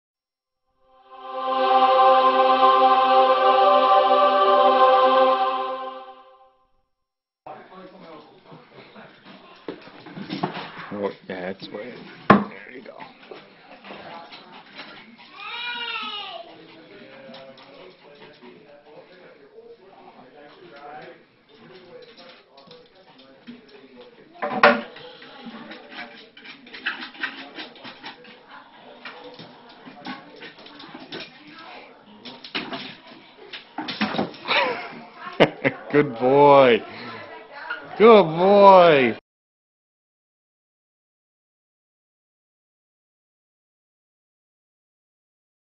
The Amazing Beast Dog
Smartest yorkie ever! Watch him solve this puzzle.